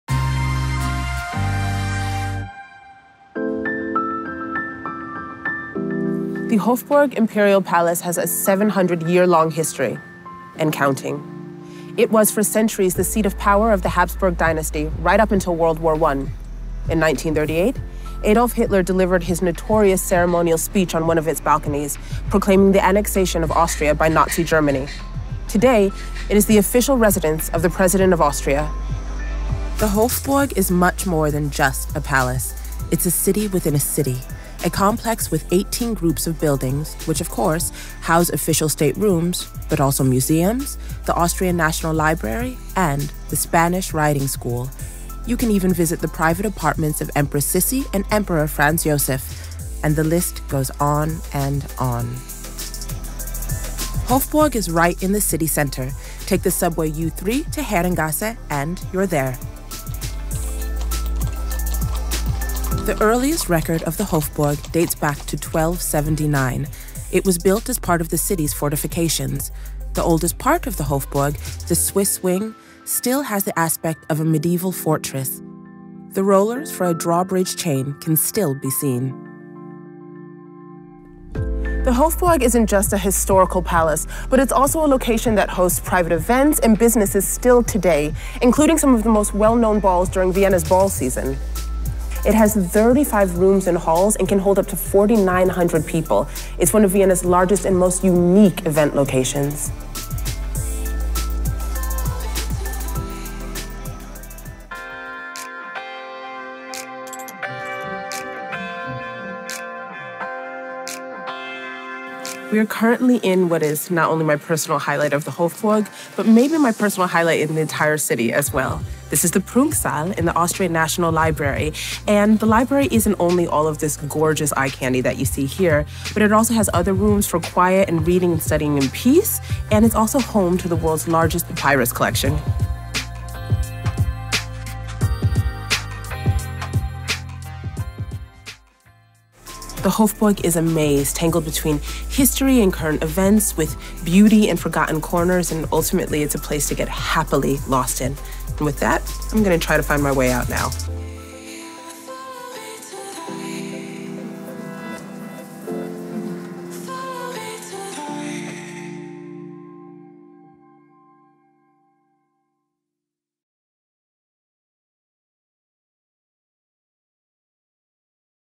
Inside the Hofburg Palace Vienna | VIENNA/NOW Sights
Take a look inside the Hofburg Palace in Vienna. In this video, Adia shows us her favourite spots in Vienna's Imperial Palace.

Topics of this video:
00:00 Intro
00:30 Inside the Hofburg Palace
01:01 History of Hofburg Palace
01:45 State Hall of the National Library (Prunksaal)

Vienna's Imperial Palace is one of the biggest palace complexes in the world. The oldest parts date to the 13th century, with construction having continued right into the 20th century. The Imperial Palace was the residence and seat of government of the Habsburg emperors until 1918. Today, it is home to numerous museums with outstanding collections, the Spanish Riding School, a congress center, the seat of the Austrian Federal President as well as the historic Heldenplatz.

For more than seven centuries, the Habsburg empire was ruled from the Hofburg Imperial Palace. Today, the Gothic Imperial Chapel, where the Vienna Boys’ Choir performs during High Mass on Sunday, is a remnant of the Imperial Palace during the Middle Ages. In the middle of the city center, you can marvel at the splendor and magnificence that made up the everyday life of the former imperial family on a visit of the private chambers and staterooms. Numerous museums and collections represent the imperial family’s passion for art.

Historically developed complex:
The Imperial Palace, which until 1918 was inhabited by the imperial family, was originally a castle built in the thirteenth century, which was extended to a splendid residence in accordance with the increasing power of the Habsburgs and the expansion of their realm. Today, the Imperial Palace houses the office of the Federal President as well as an important congress center and numerous art collections. Historic Heldenplatz, once planned as an extensive imperial forum, is dominated by the monumental equestrian statues of Prince Eugen and Archduke Karl. This spot affords the best view of the Imperial Palace as well as the adjacent Volksgarten.

Credits:
Presenter: Adia Trischler
Producer: Alex Haberfellner
Camera: Bernhard Popovic, Andreas Graf
Editing, Animation: Thomas Scharf
Colors: 61 colorgrading,
Audio: Overdub, Gregor Rašek
Produced by Unit